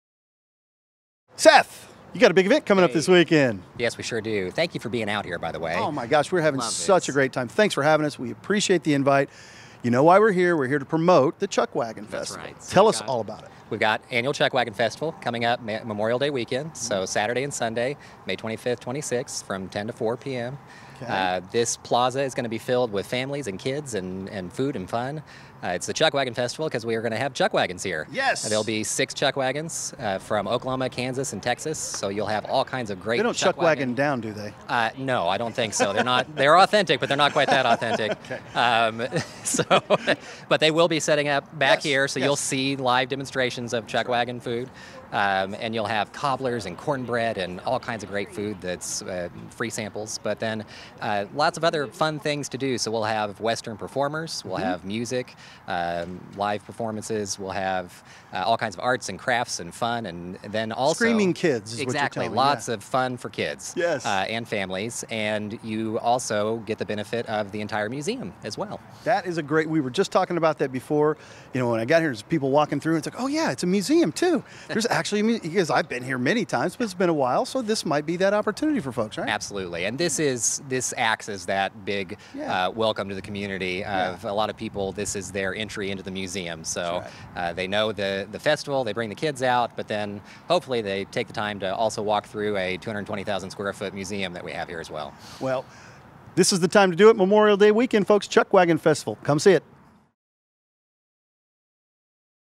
Chuck Wagon Festival rolls into OKC for Memorial Day Weekend
The National Cowboy & Western Heritage Museum will be hosting the annual Chuck Wagon Festival this weekend.